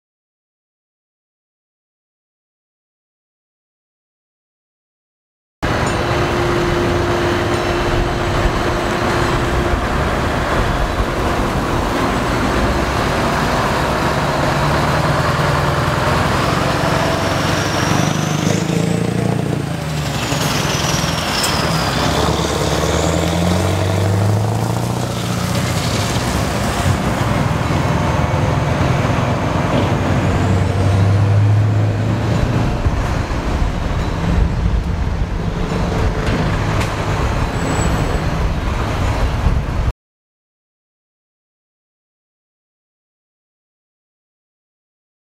Peter Møller - Scania R500 V8 (short clip)
Taking off at Åbenrå Port in Denmark.